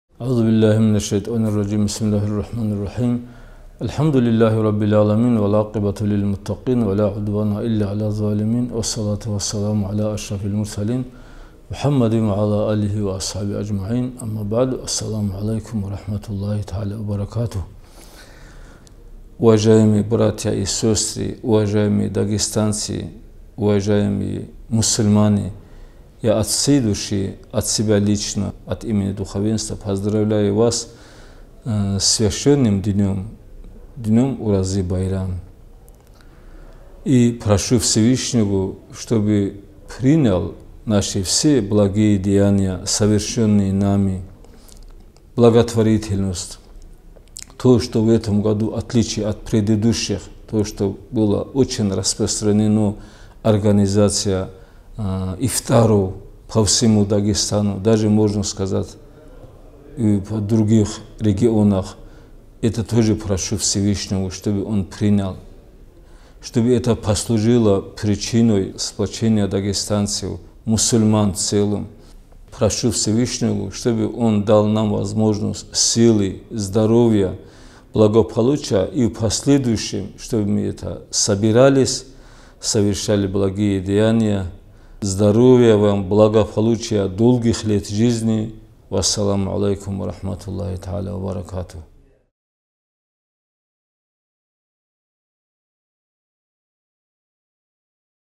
Муфтий РД шейх Ахмад Афанди поздравил верующих с праздником Ураза Байрам.
Муфтий Дагестана Шейх Ахмад Афанди поздравил мусульман с праздником Ураза-Байрам. Духовный лидер республики в своей речи обратился с мольбой ко Всевышнему, чтобы все благие деяния верующих в месяце Рамадан были приняты.

Муфтий региона также пожелал соотечественникам здоровья, силы и благополучия и последующем совершать больше благих деяний.